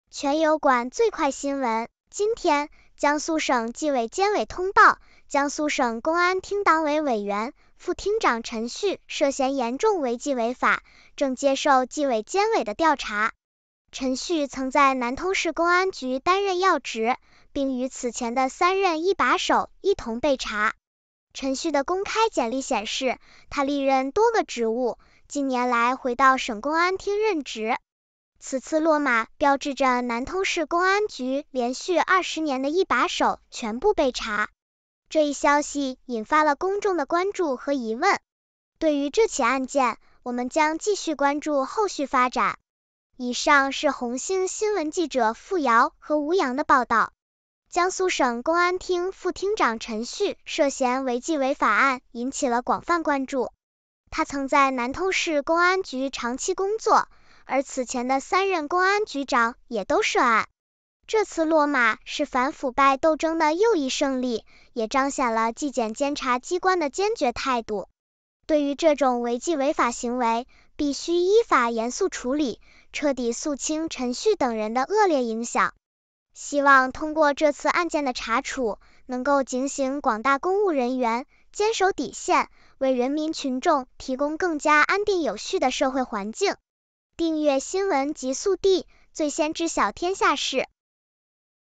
中国新闻10月28日14时：江苏省公安厅副厅长陈旭被查，曾任南通市公安局局长，该局4任“一把手”均落马
【播报】今天，江苏省纪委监委通报，江苏省公安厅党委委员、副厅长陈旭涉嫌严重违纪违法，正接受纪委监委的调查。陈旭曾在南通市公安局担任要职，并与此前的三任“一把手”一同被查。陈旭的公开简历显示，他历任多个职务，近年来回到省公安厅任职。此次落马标志着南通市公安局连续20年的“一把手”全部被查。这一消息引发了公众的关注和疑问。对于这起案件，我们将继续关注后续发展。以上是红星新闻记者付垚和吴阳的报道。【点评】江苏省公安厅副厅长陈旭涉嫌违纪违法案引起了广泛关注。他曾在南通市公安局长期工作，而此前的三任公安局长也都涉案。这次落马是反腐败斗争的又一胜利，也彰显了纪检监察机关的坚决态度。对于这种违纪违法行为，必须依法严肃处理，彻底肃清陈旭等人的恶劣影响。希望通过这次案件的查处，能够警醒广大公务人员，坚守底线，为人民群众提供更加安定有序的社会环境。